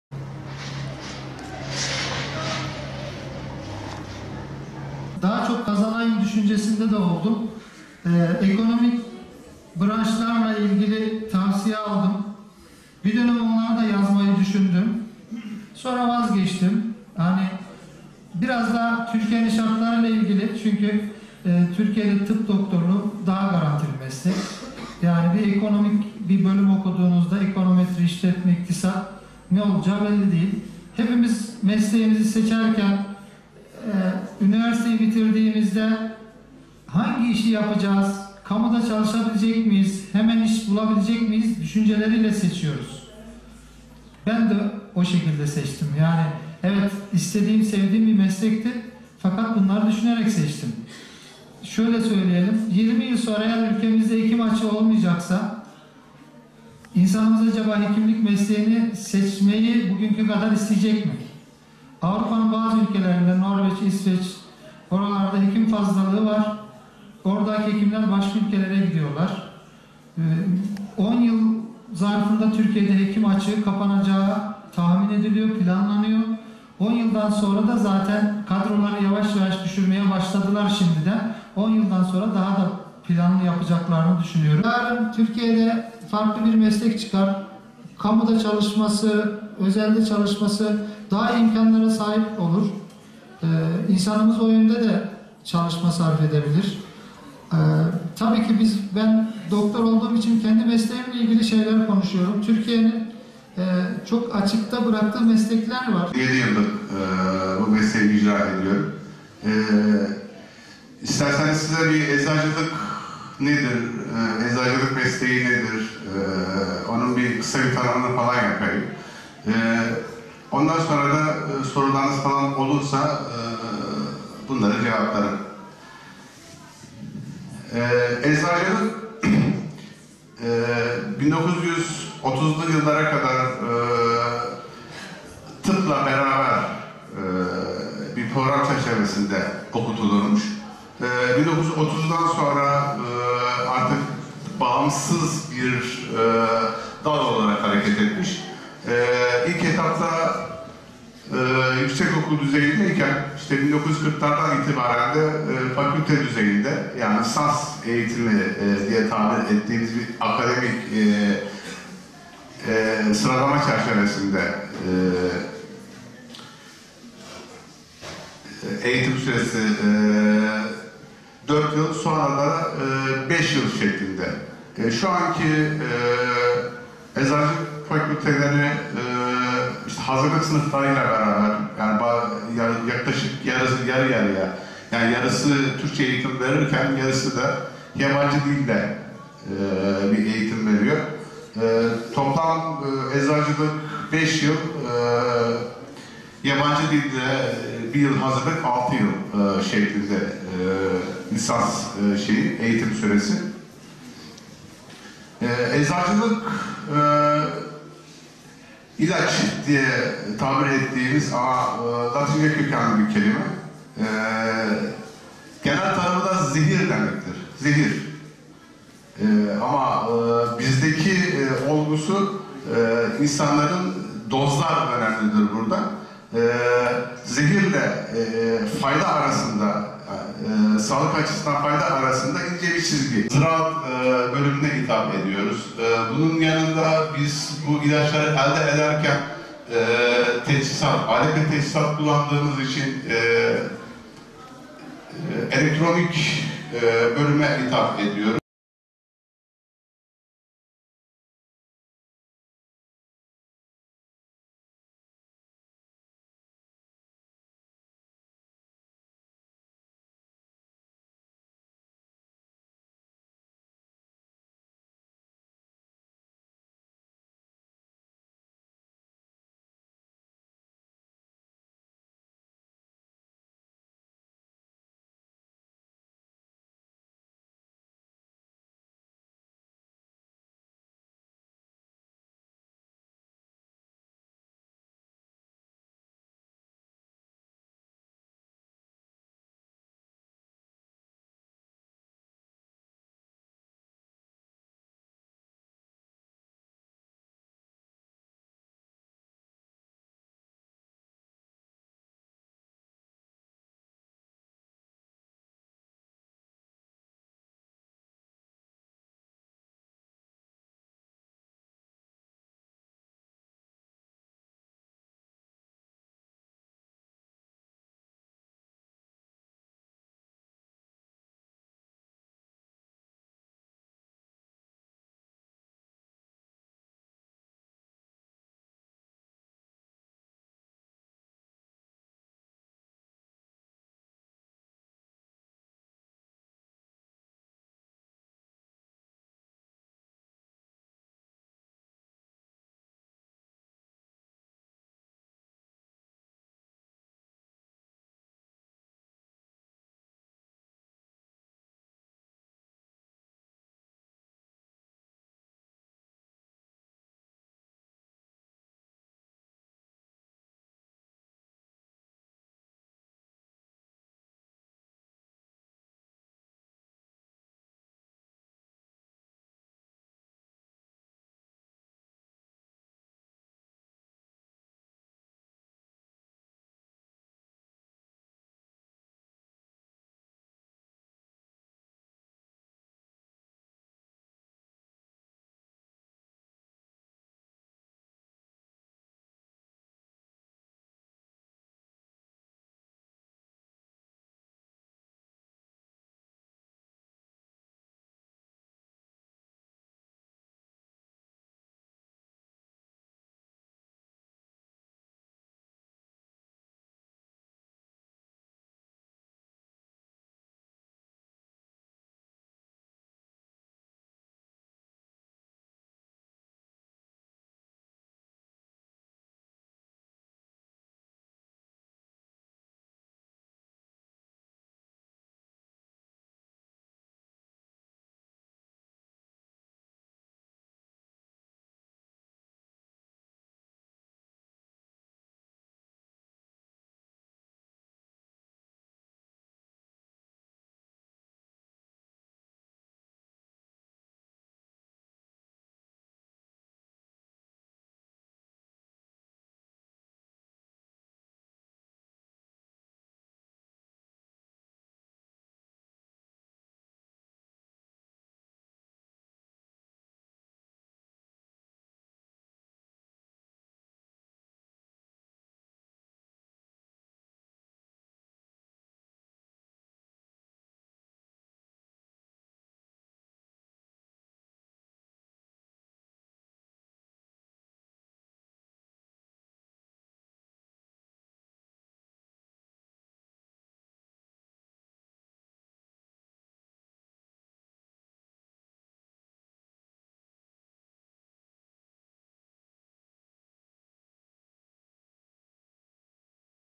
Öğrencilere kariyer semineri
Nizip İbn-i Sina Mesleki ve Teknik Anadolu Lisesi tarafından düzenlenen “Kariyer Günleri” etkinliğine katılan Nizip Devlet Hastanesi İç Hastalıkları Uzmanı Doktor Enver Aytemiz ve Eczacı Mehmet Ali Aybar Babacan öğrencilere mesleğe nasıl başladıklarını ve meslekleri ile ilgili merak edilenleri anlattı.

Okulun konferans salonunda gerçekleşen programda meslekleri konusunda ve akademik başarı konusunda öğrencilere kazanımlarını anlatan Doktor Enver Aytemiz ve Eczacı Mehmet Ali Babacan ayrıca öğrencilerden gelen soruları da yanıtladı.

Doktor Enver Aytemiz ve Eczacı Mehmet Ali Babacan daha sonra meslek seçiminin önemi hakkında da sunum yaptı.